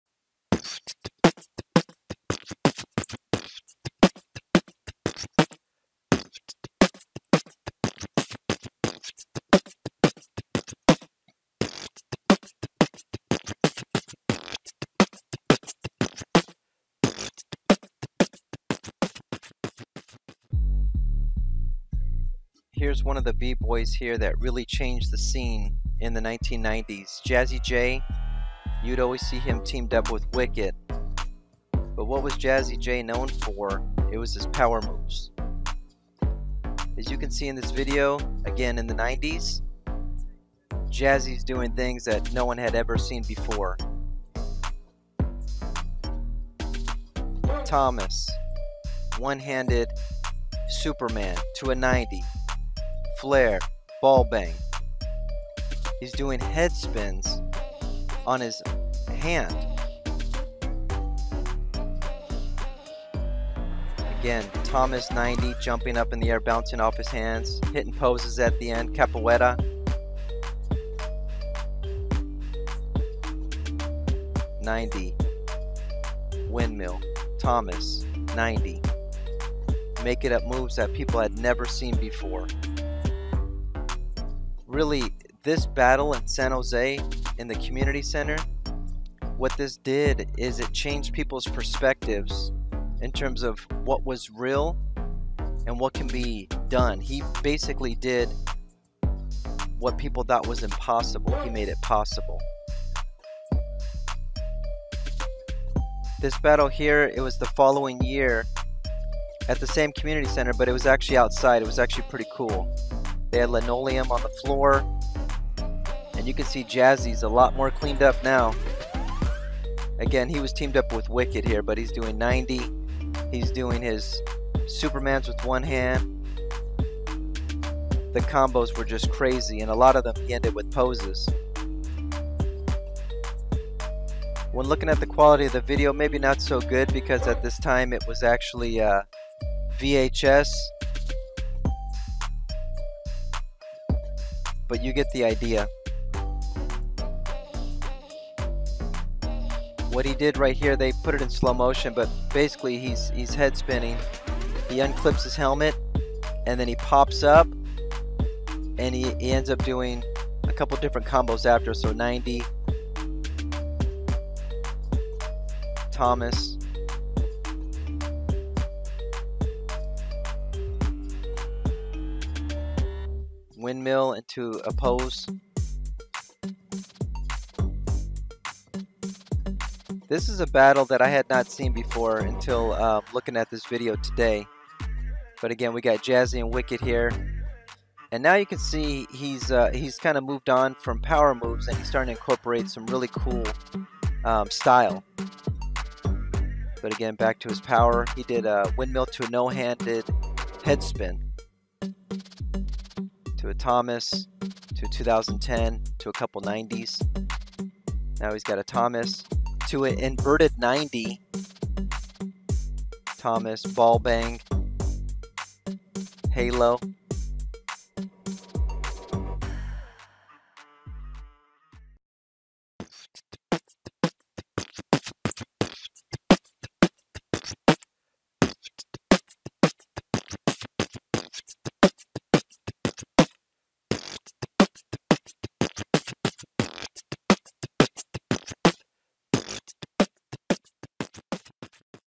Breaking it Down - Giving Back (Video #1 Jazzy J)
Jazzy J broke out on the scene in the 1990's. He was known for his insane power moves and difficult combos. Jazzy J changed the b-boy game and made fellow break-dancers better by making the impossible look easy.